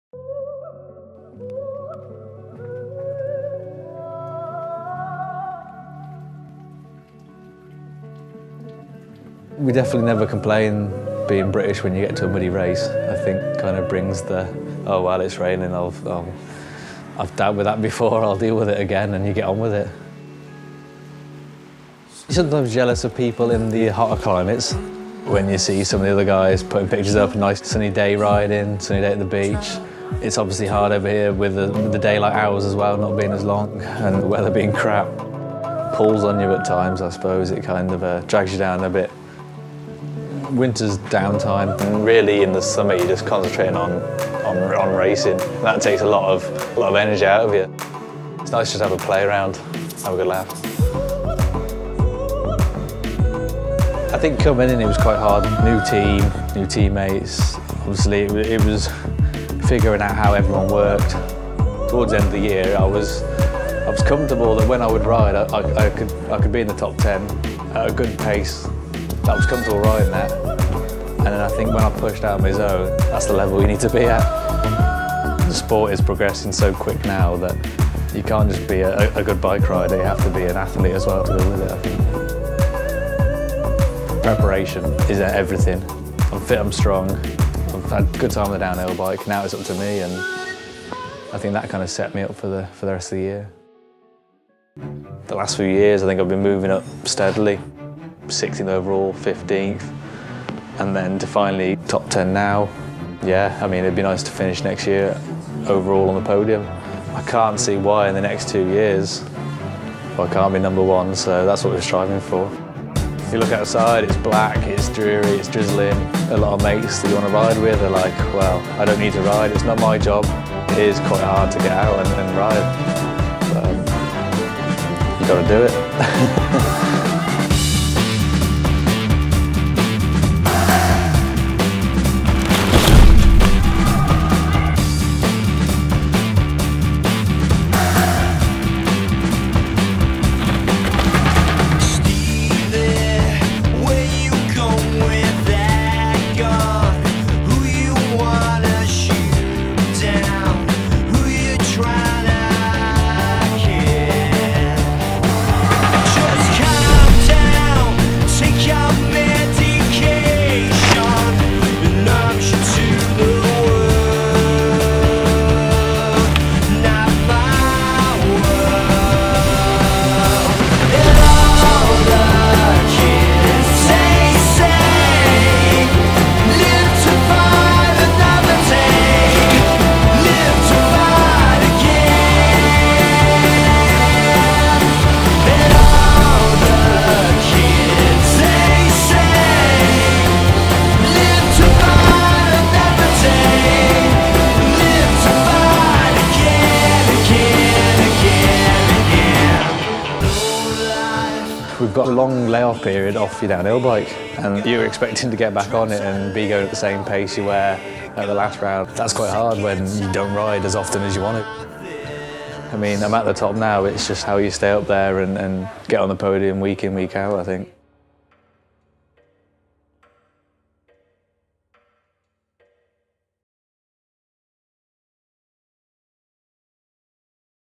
Reflections, Part One - Preparation is Everything
Matt Simmonds reflects on his first season on the Madison Saracen Factory Team, during which he claimed a second place at the Méribel World Cup round, finishing 7th overall in the World Cup standings